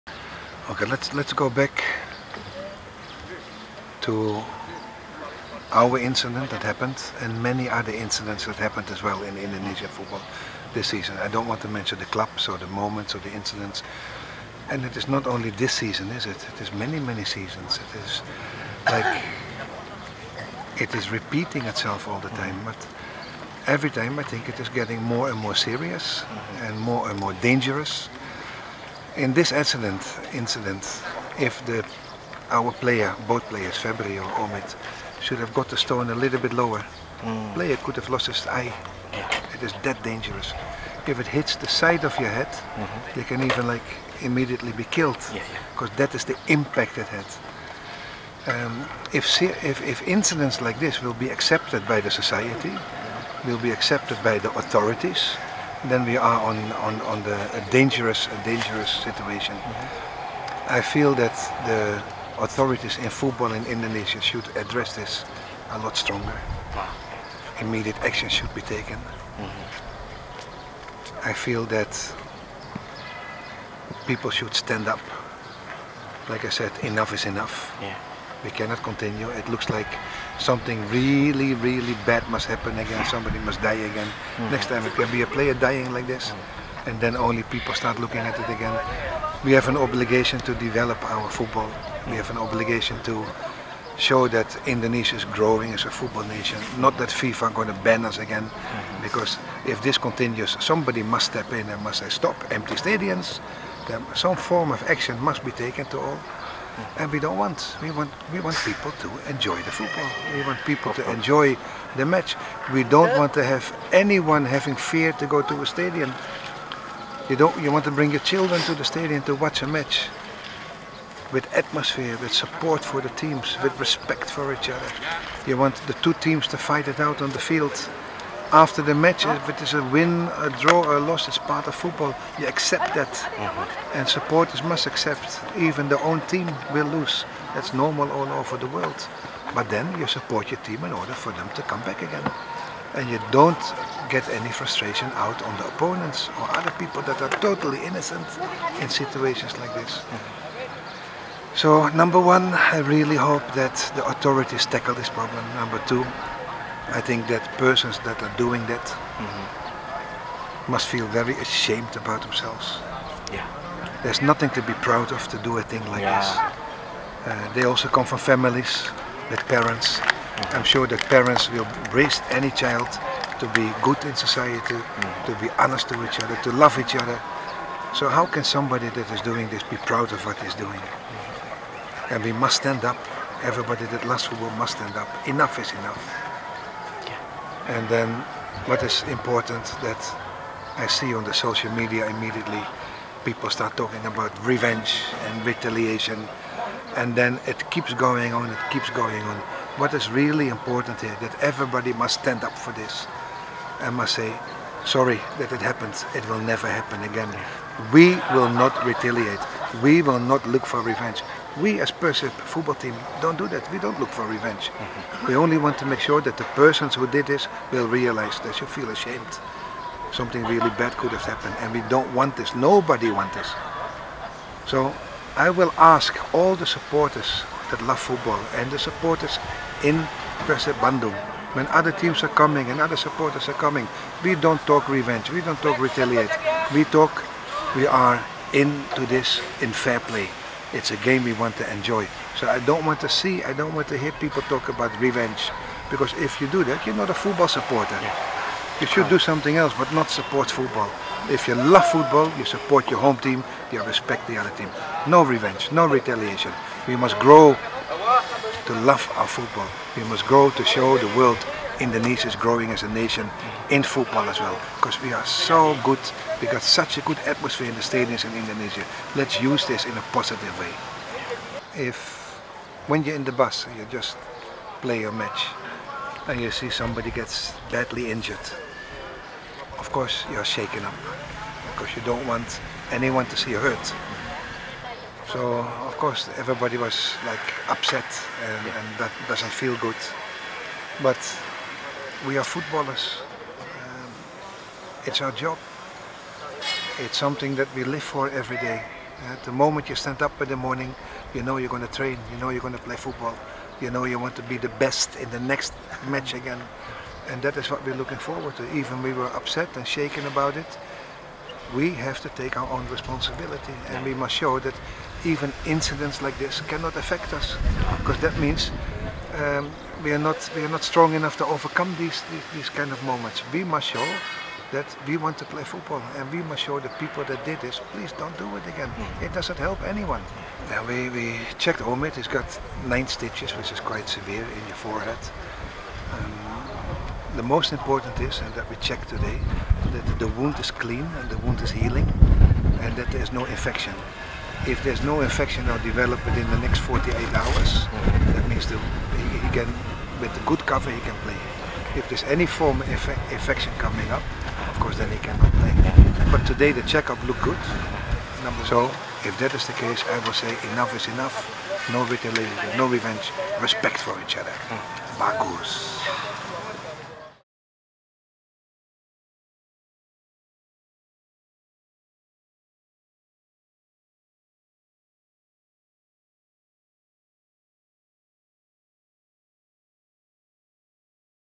Robert: Enough is Enough. Setop Kekerasan Dalam Sepakbola!
Pernyataan lengkap pelatih PERSIB, Robert Alberts, terkait insiden penyerangan ke bus PERSIB. Mari, sama-sama mendukung gerakan StopKekerasanDalamSepakbola dimulai dari diri kita sendiri dengan tidak melakukan penyerangan meskipun dengan aksi balasan. Cukup sudah berhenti di sini.